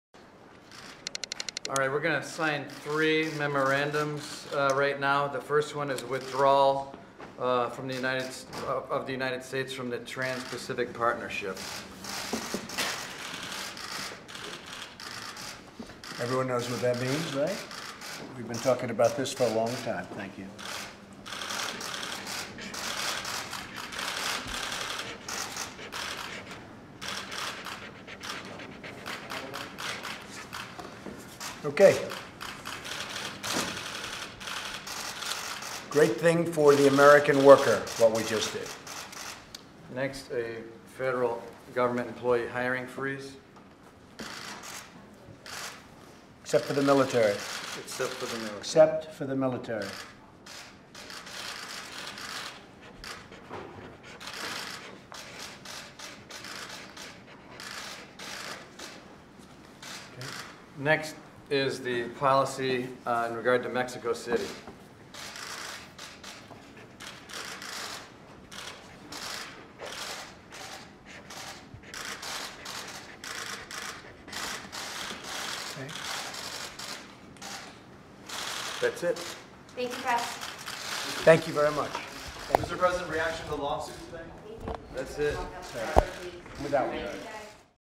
Trump Withdraws U.S. From TPP Through Executive Order 4th Day In Office
Trump Withdraws U.S. From TPP (2017)

President Donald J. Trump withdraws the United States from the Trans-Pacific Partnership (TPP) via executive order on his fourth day in office, January 23rd 2016

Donald Trump
President Trump
Trump Presidency
Trans-Pacific Partnership
TPP